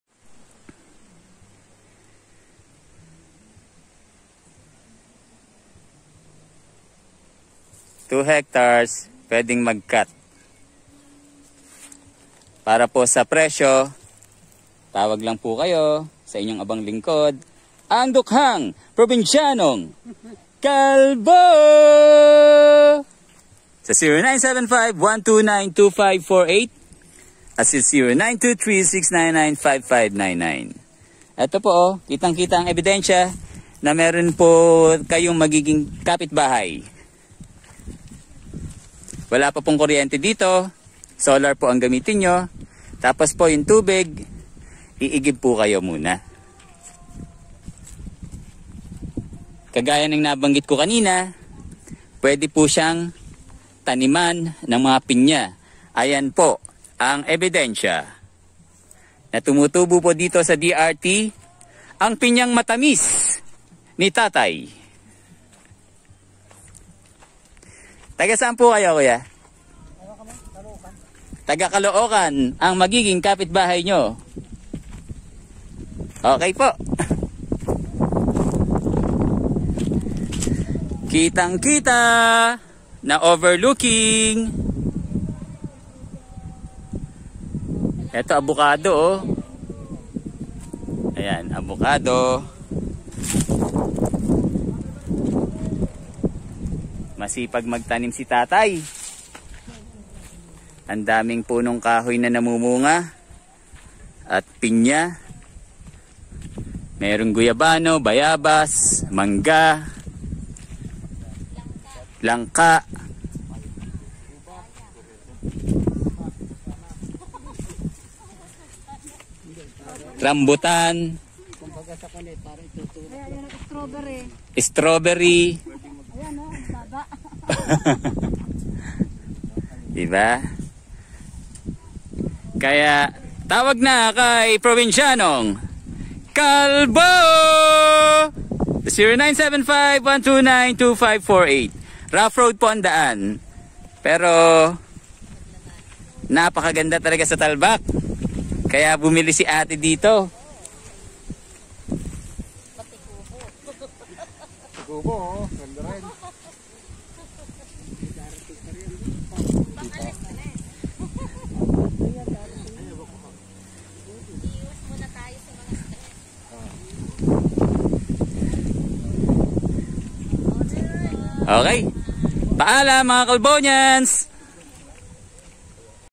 DRT Overlooking Farm Lot Na Rights Only, Pwede Po Ang Small Cut, Rough Road Pa Po Sa Ngayon, GANDA 😊
Kindly watch @probinsyanongkalbochannel